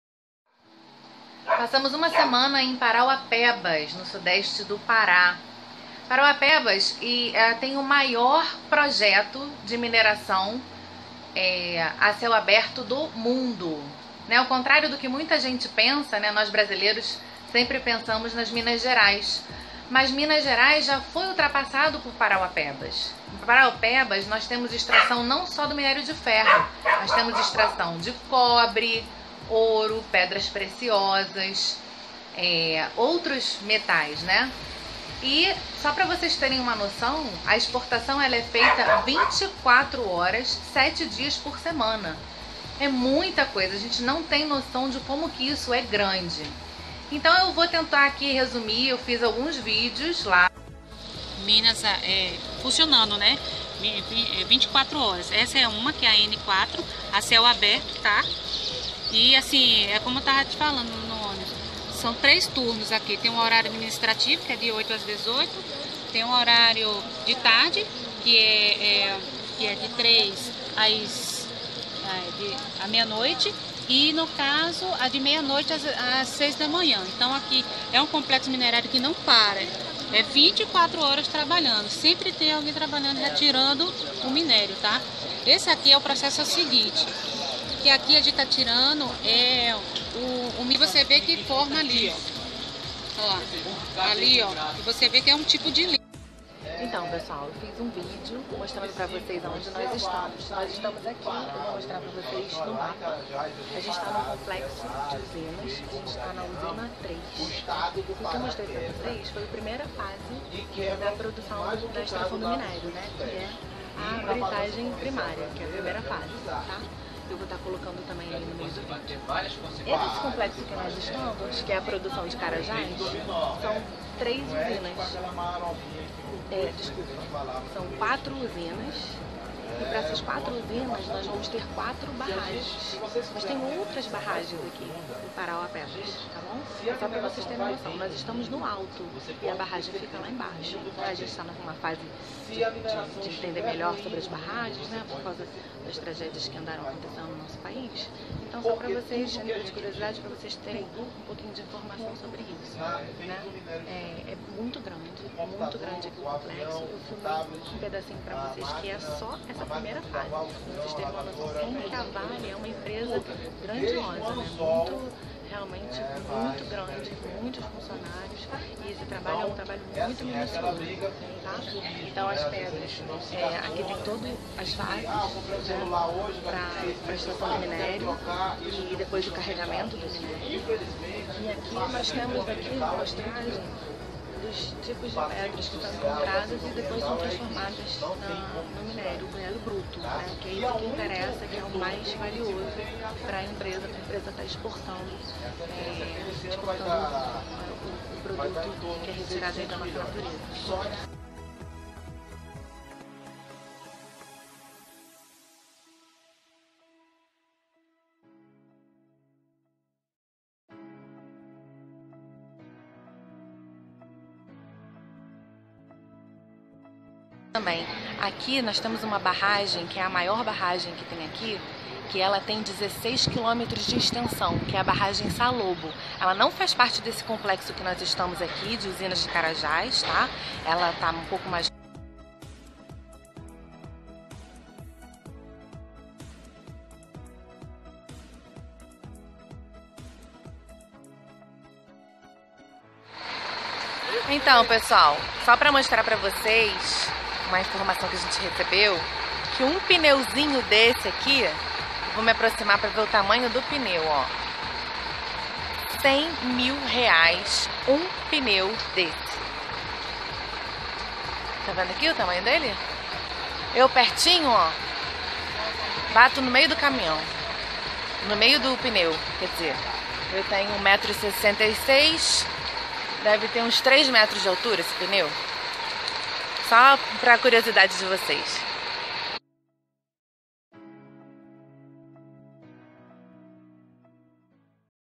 Mina N4 Mineração de Carajas - Pará
Mina N4 Mineração de Carajas

Conhecendo um pouco sobre a extração de minério na Produção de Carajás - Mina N4 - em Parauapebas
Você sabia que lá existe uma barragem com 16 km de extensão?
E que um pneu de caminhão pode custar R$100.000???
E que exportamos minério de ferro 24h, 7 dias por semana?
Gente, nosso país é muito rico!!!
Tia Priscila Brum conta tudo pra vocês!!

Informações e Reservas: Lenhatour Turismo